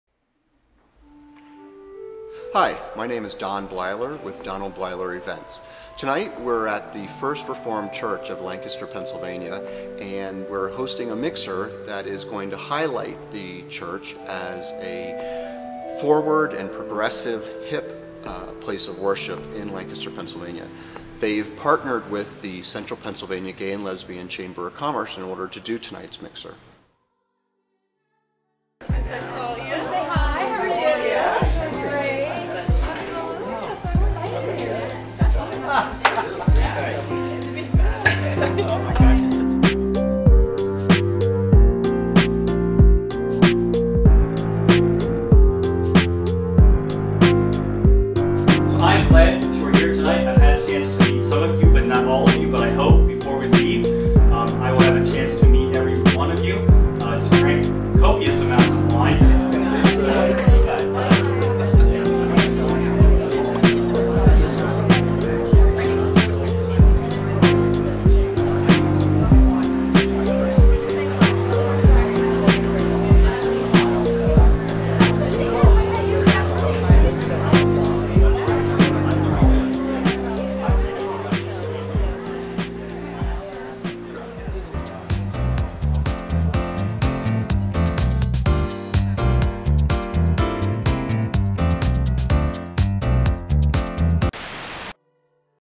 First Reformed Church CPGLCC Mixer
First Reformed Church Cocktail Mixer in collaboration with CPGLCC